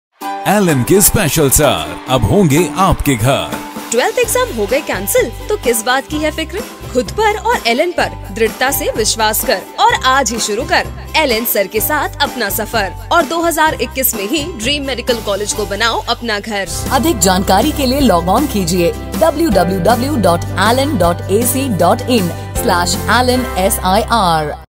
ALLEN SIR 2.0 | The Exclusive, Selective & Interactive Revision to ace NEET2021 | NEET-UG Revision
📣 With the cancellation of the Class 12th Board Exam, here is an excellent opportunity to assure your selection in NEET-UG 2021.
➡️ Join ALLEN's Special Selective Interactive Revision (SIR 2.0), this specially crafted course is an amazing opportunity for you to get the much-needed revision to ace NEET-UG 2021
👉 Course starting from 10 June 2021
👉 Mode: Online | Course Duration: 50 Days | Form Fee: Rs 100 | Course Fee: Rs 9000+GST
✔ Get Recorded Lectures of Selective Topics.
✔ Daily Worksheet of 15 to 30 most probable questions.
✔ Daily Live Discussion Class on given worksheets
✔ 3 Part test on NEET Pattern every 15 days and 2 ALLEN NEET (Full Syllabus) with classroom students.